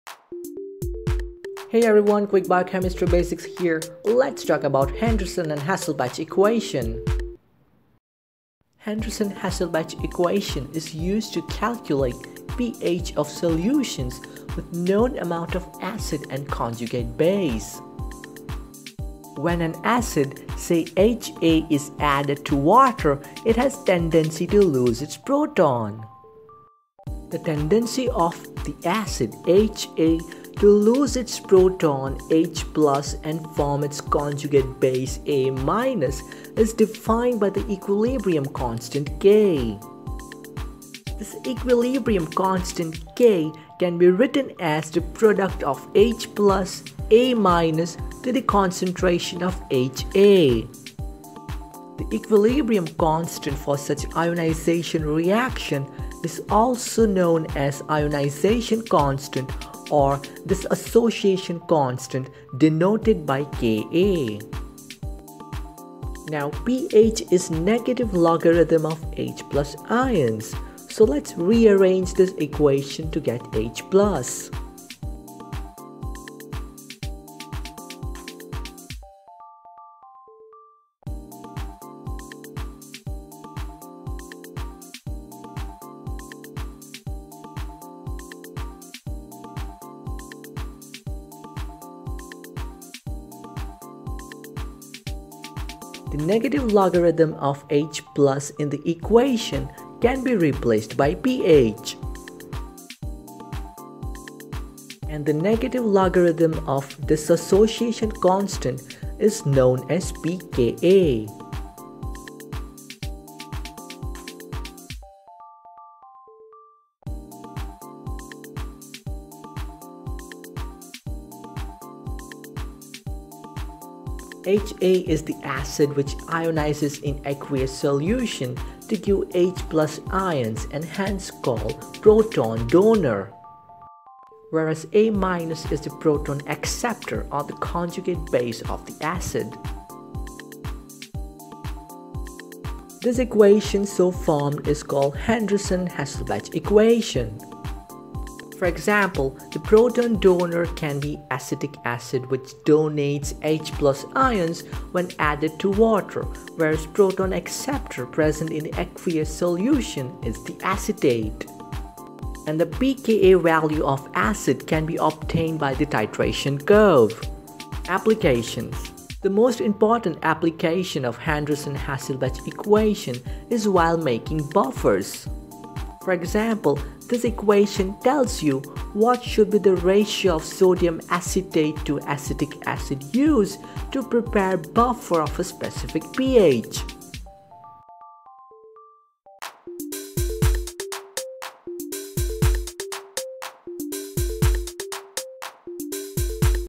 Henderson Hasselbalch equation.
Henderson Hasselbalch equation describes the derivation of pH as a measure of acidity (using pKa, the negative log of the acid dissociation constant) in biological and chemical systems. Henderson–Hasselbalch equation is also useful for estimating the pH of a buffer solution and finding the equilibrium pH in acid-base reactions (it is widely used to calculate the isoelectric point of proteins).